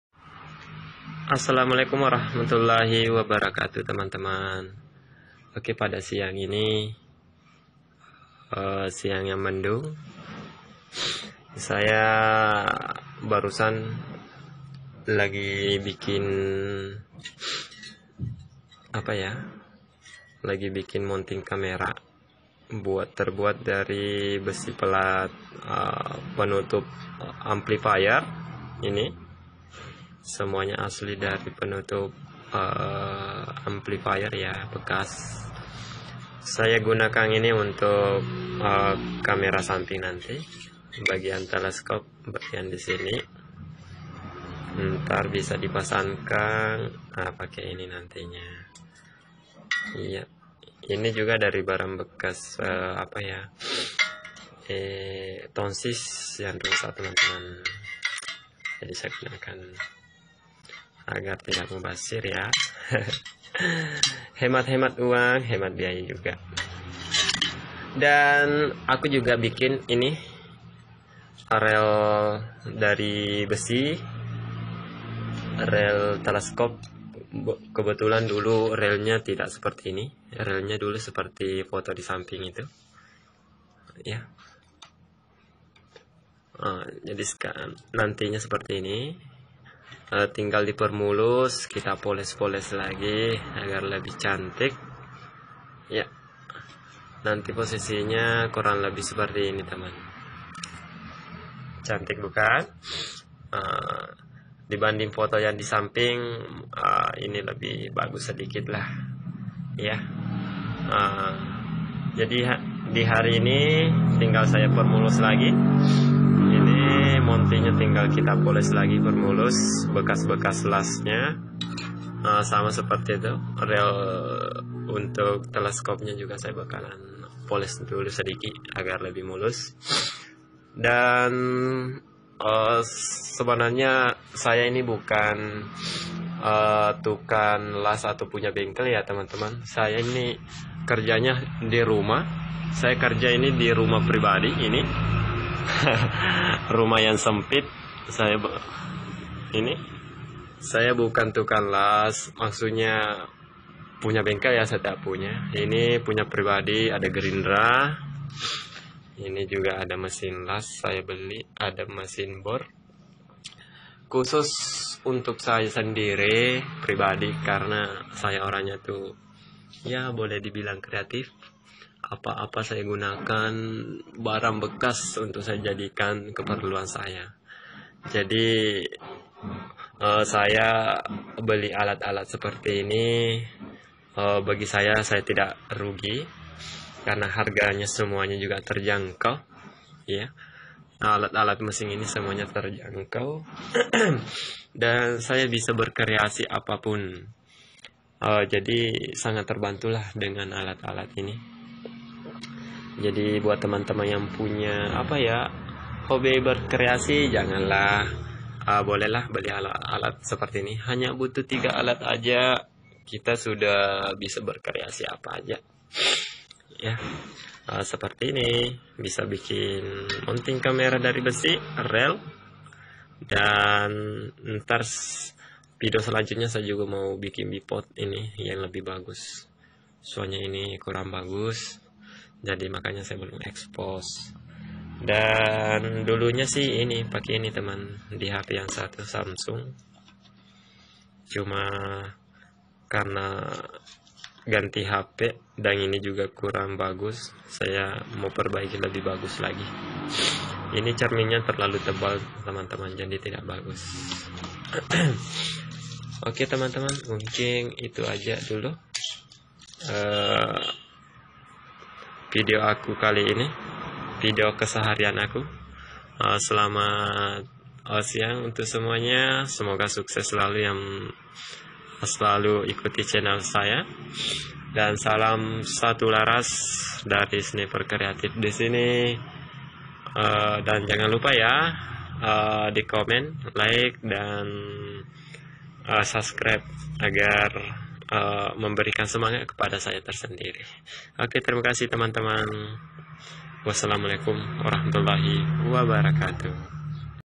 Membuat monting kamera dan rel teleskop ala(sniper kreatif)
Aku membuat monting kamera dan rel teleskop ini untuk di pakai pribadi,,,bahan yang aku gunakan pun tidak susah dicari karena kebanyakan dari barang bekas,,, dan buat teman 2 muda2han bisa jadi inspirasi,,, makasih banyak yang Uda subscribe ya,,,,